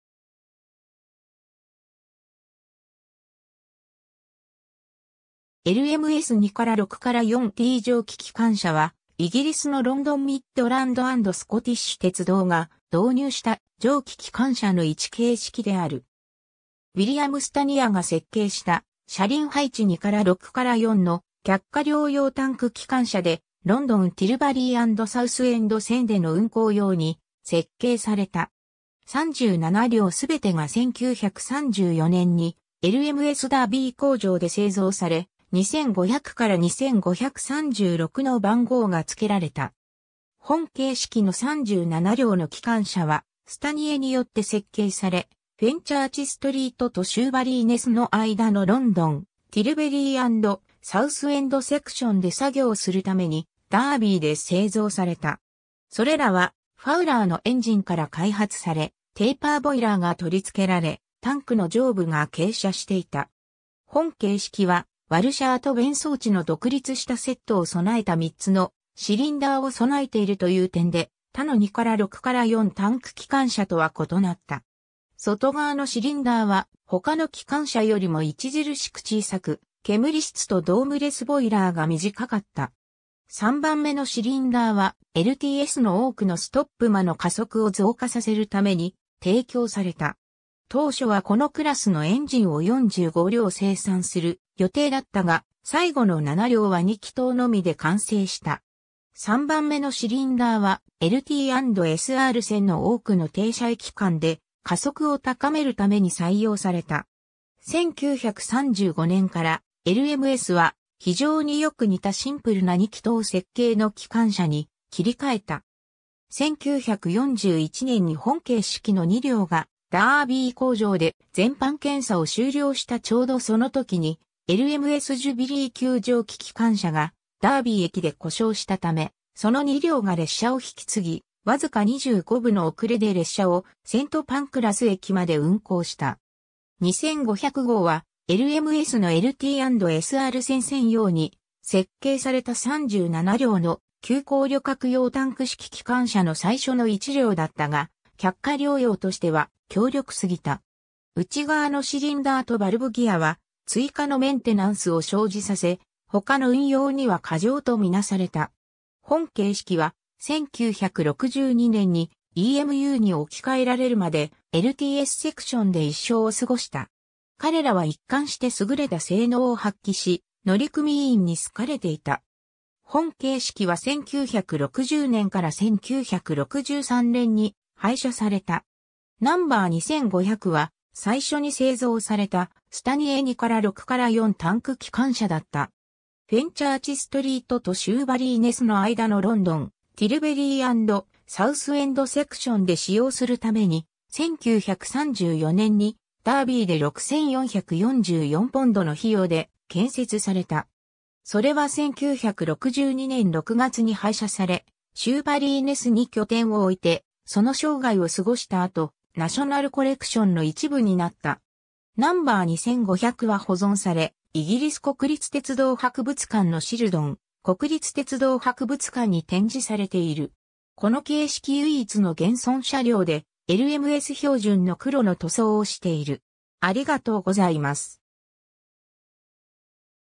LMS 2-6-4T蒸気機関車 (スタニア・3気筒)
LMS 2-6-4T蒸気機関車（LMS 2-6-4Tじょうききかんしゃ）は、イギリスのロンドン・ミッドランド・アンド・スコティッシュ鉄道（LMS）が導入した蒸気機関車の1形式である。
ウィリアム・スタニアが設計した車輪配置 2-6-4（1C2）の客貨両用タンク機関車で、ロンドン・ティルバリーアンドサウスエンド線(LT&S.R線)での運行用に設計された。
37両すべてが1934年にLMSダービー工場で製造され、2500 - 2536の番号が付けられた。
 本形式の37両の機関車は、スタニエによって設計され、フェンチャーチストリートとシューバリーネスの間のロンドン、ティルベリー＆サウスエンド（LTS）セクションで作業するためにダービーで製造された。
それらはファウラーのエンジンから開発され、テーパーボイラーが取り付けられ、タンクの上部が傾斜していた。
 本形式は、ワルシャート弁装置の独立したセットを備えた3つのシリンダーを備えているという点で、他の2-6-4タンク機関車とは異なった。
外側のシリンダーは他の機関車よりも著しく小さく、煙室とドームレスボイラーが短かった。
3番目のシリンダーは、LTSの多くのストップ間の加速を増加させるために提供された。
当初はこのクラスのエンジンを45両生産する予定だったが、最後の7両は2気筒のみで完成した。
3番目のシリンダーは、LT＆S.R線の多くの停車駅間で加速を高めるために採用された。
1935年から、LMS は非常によく似たシンプルな2気筒設計の機関車に切り替えた。
1941年に本形式の2両がダービー工場で全般検査を終了したちょうどそのときに、LMSジュビリー級蒸気機関車がダービー駅で故障したため、その2両が列車を引き継ぎ、わずか25分の遅れで列車をセントパンクラス駅まで運行した。
 2500号は、LMSのLT&S.R線専用に設計された37両の急行旅客用タンク式機関車の最初の1両だったが、客貨両用としては強力すぎた。
内側のシリンダーとバルブギアは、追加のメンテナンスを生じさせ、他の運用には過剰と見なされた。
 本形式は、1962年にEMUに置き換えられるまで、LTSセクションで一生を過ごした。
彼らは一貫して優れた性能を発揮し、乗組員に好かれていた。
本形式は1960年から1963年に廃車された。
 No. 2500は、最初に製造されたスタニエ2-6-4タンク機関車だった。
 フェンチャーチストリートとシューバリーネスの間のロンドン、ティルベリー＆サウスエンド（LTS）セクションで使用するために、1934年にダービーで6,444ポンドの費用で建設された。
それは1962年6月に廃車され、シューバリーネスに拠点を置いてその生涯を過ごした後、ナショナルコレクションの一部になった。
 No. 2500は保存され、イギリス国立鉄道博物館のシルドン国立鉄道博物館に展示されている。
この形式唯一の現存車両で、LMS標準の黒の塗装をしている。